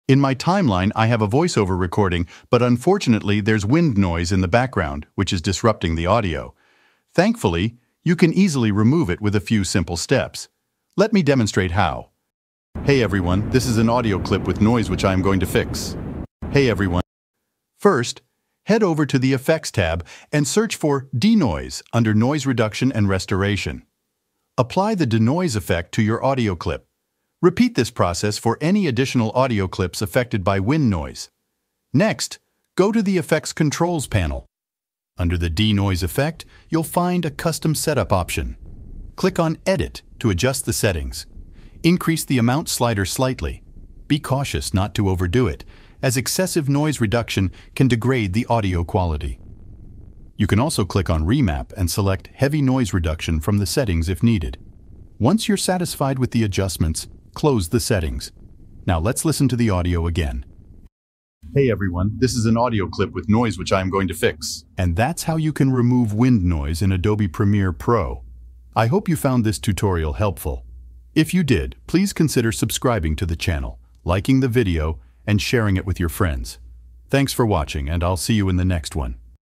How to Remove Wind Noise in Premiere Pro
In this video you learn how to fix wind noise in video with Premiere Pro.